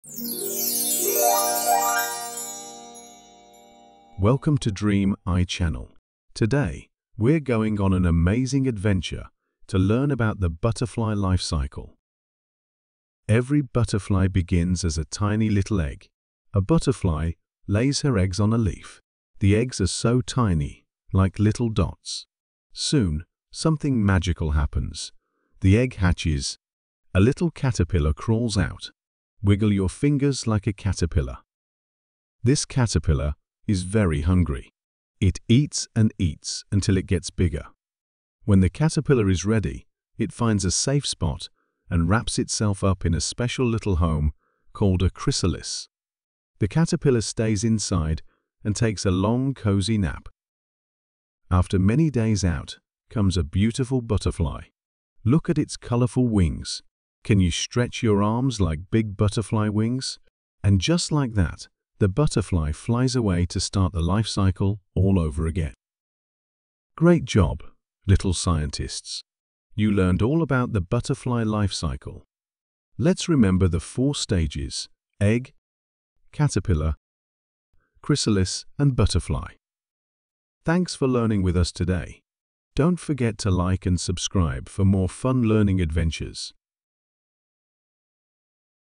Butterfly Life Cycle Vocabulary
Theme: Growing and Changing
Teach young learners about the butterfly life cycle.